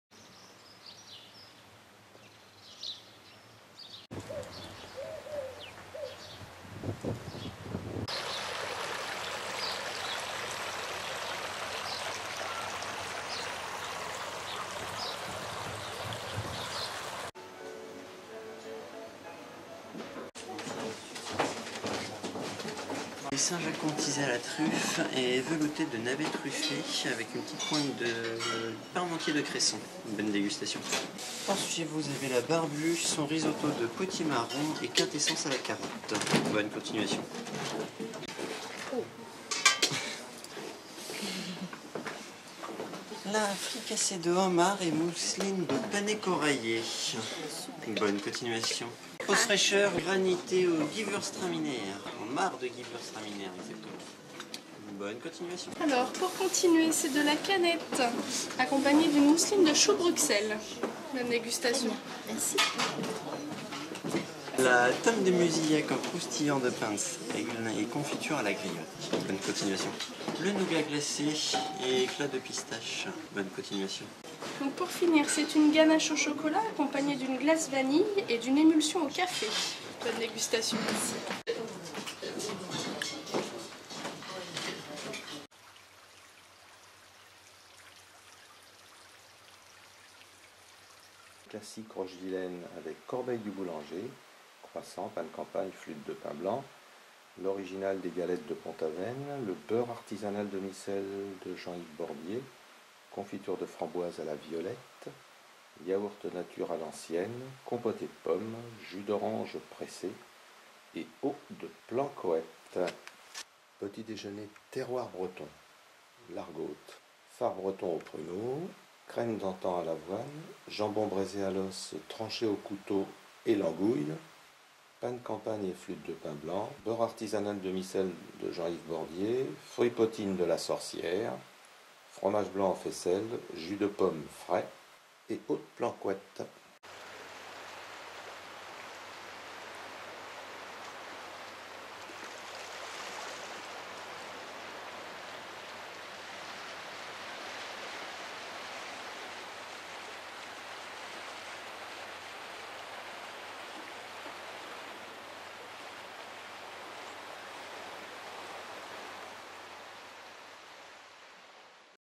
Déjeuner au Domaine de Rochevilaine * - 11 février 2012
Site, accueil, qualité de la table, chambres avec vue sur mer, bref que du bonheur à faire escale au Domaine de Rochevilaine dont Patrice Caillault assure les destinés culinaires depuis plus de 20 ans. Le menu Confiance associe au forfait vins au verre à 25 € 00 est une excellente approche.